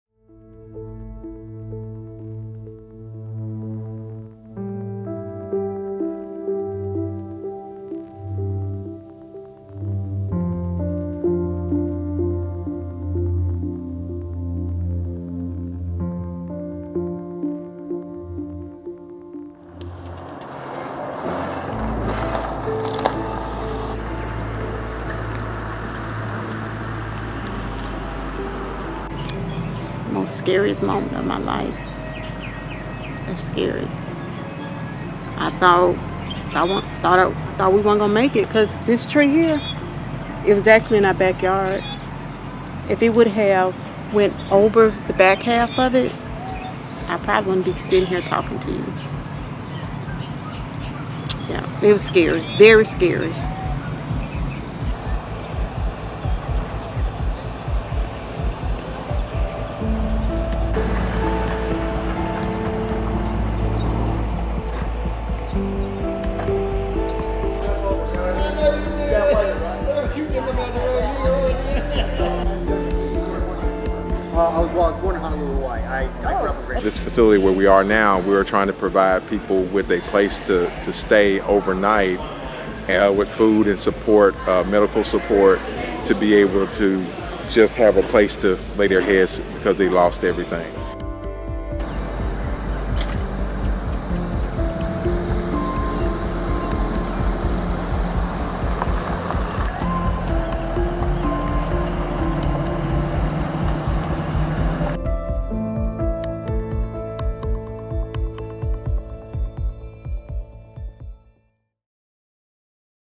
"Le moment le plus effrayant de ma vie" : le Mississippi dévasté par des tornades | AFP
L'Etat du Mississippi était sous le choc samedi après le passage de tornades dévastatrices dans cet Etat du sud des Etats-Unis, qui ont fait au moins 25 morts, notamment dans la petite ville de Rolling Fork.